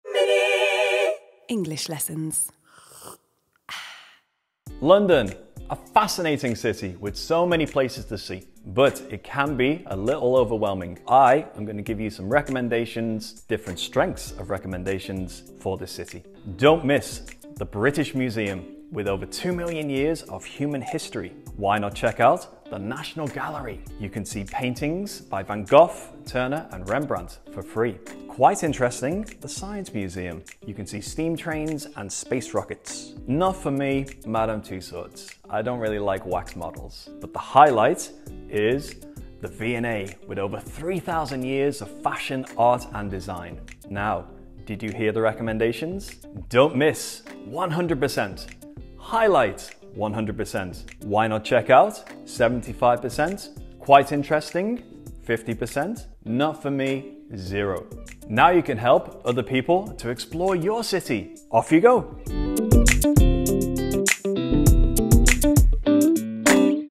Things to do in London - a Mini English Lesson
Learn how to give advice with these different levels of recommendation... and get some good London sightseeing advice at the same time 💂!
Find out more about online classes with the world's English experts